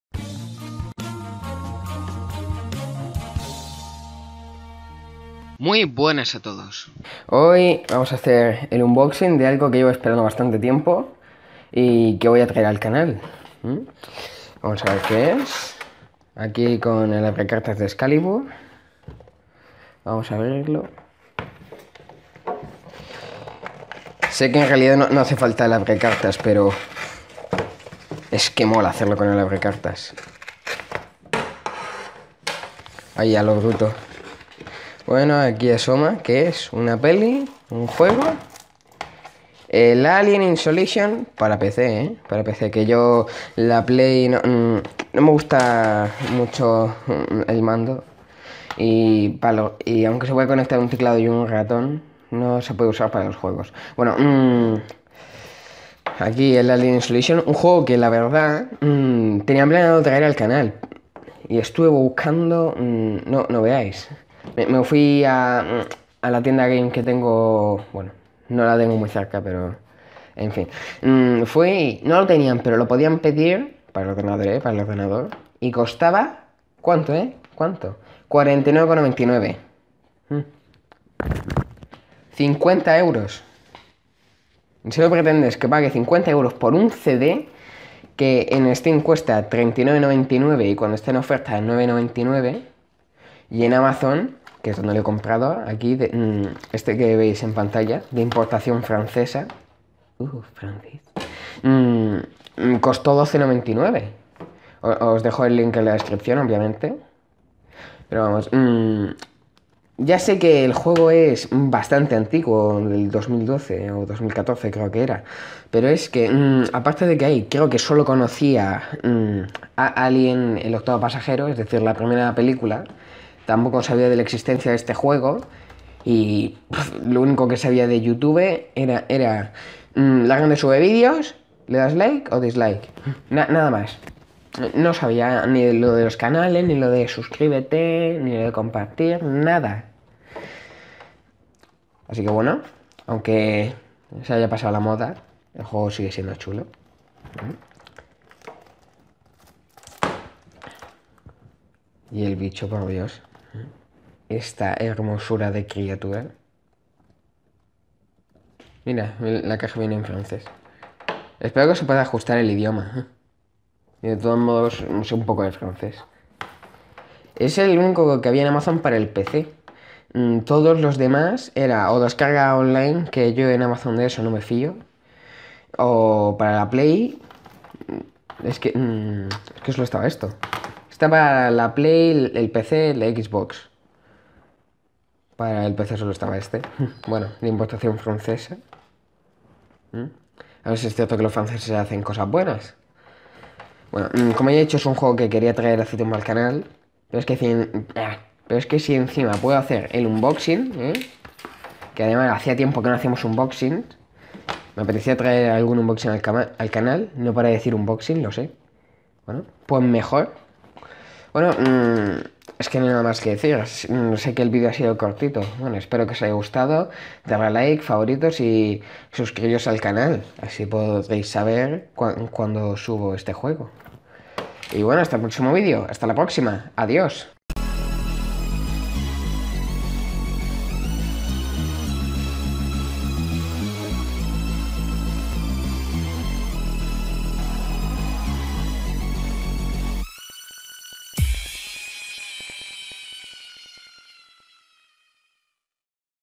unboxing alien isolation para PC
nuevo juego para el cnal de una de  mis sagas favoritas Alien

link de compra